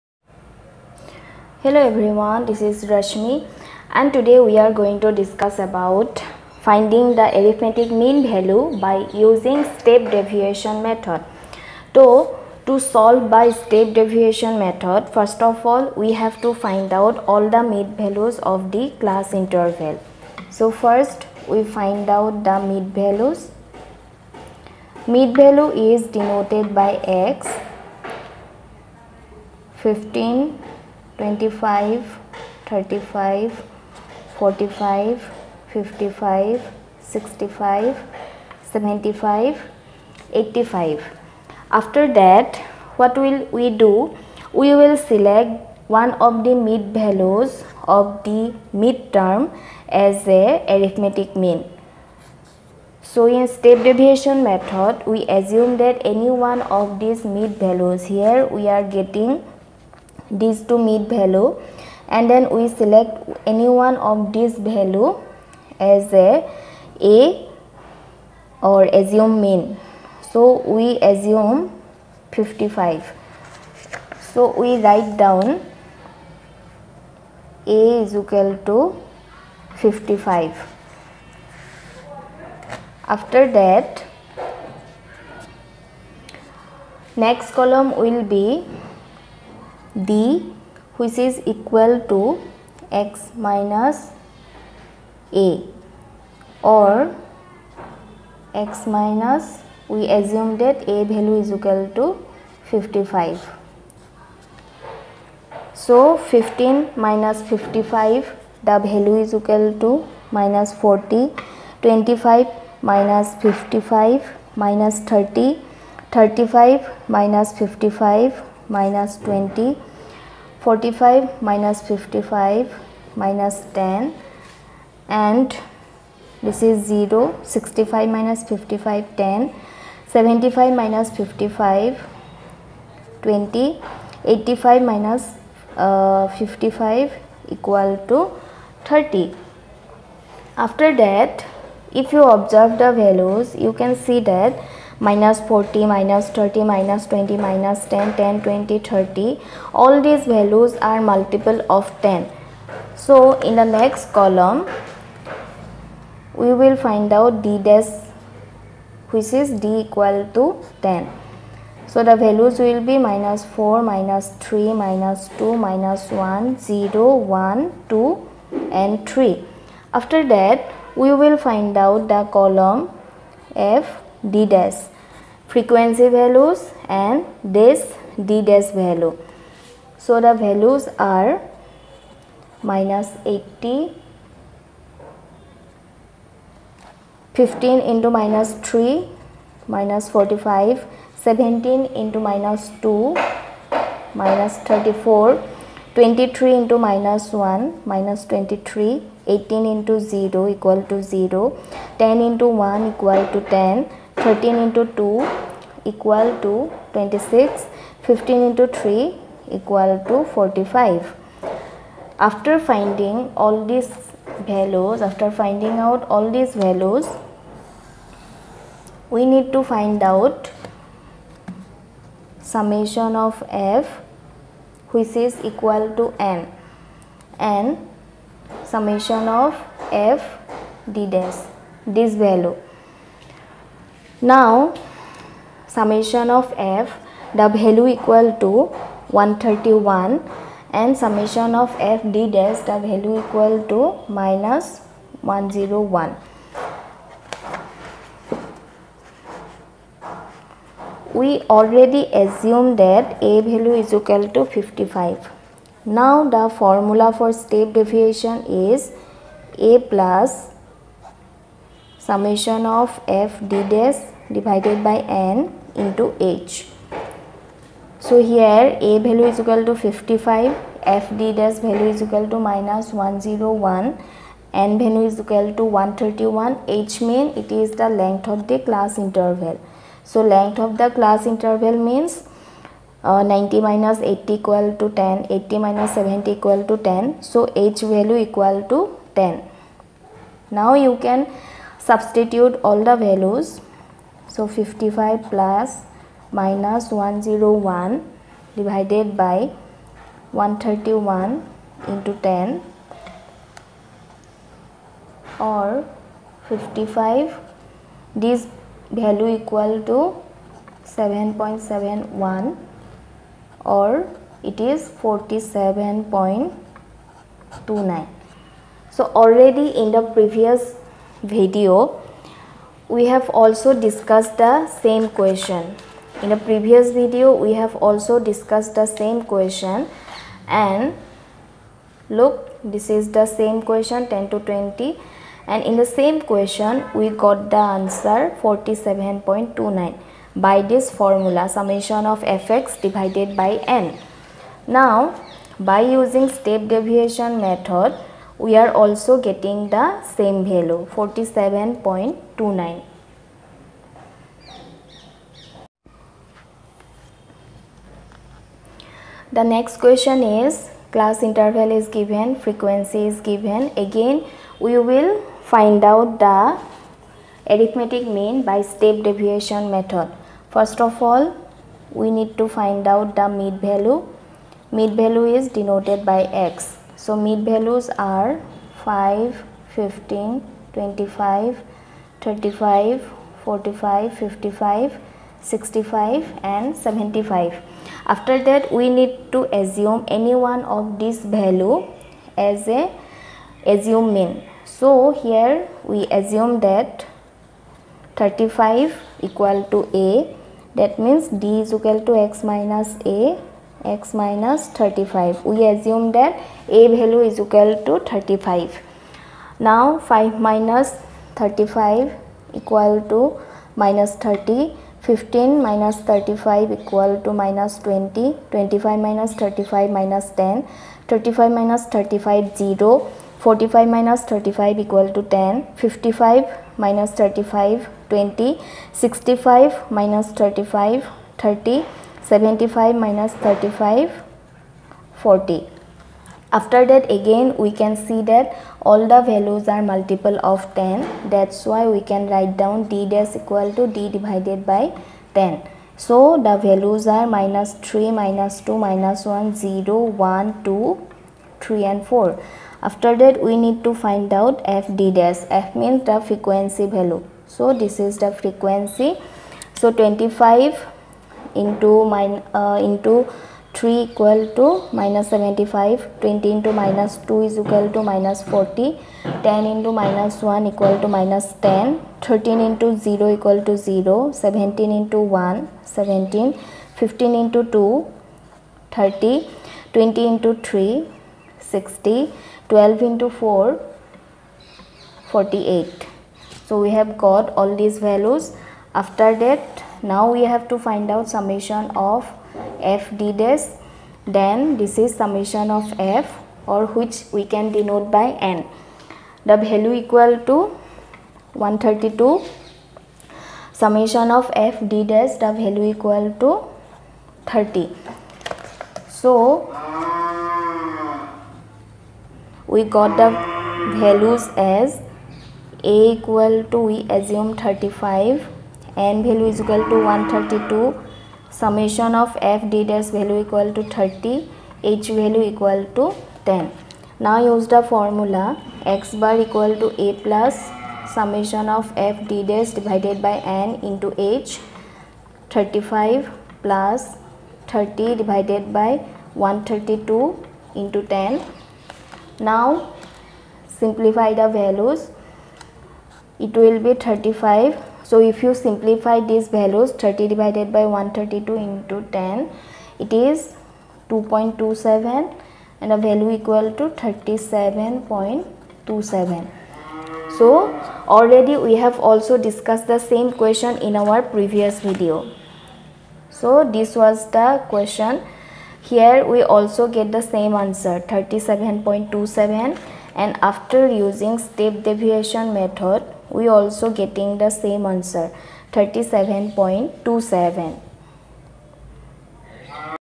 Step deviation method | How to calculate Arithmetic mean value by step deviation method
Measure of Central tendency.
What is Arithmetic mean.
How to calculate Arithmetic mean.
How to calculate Mean value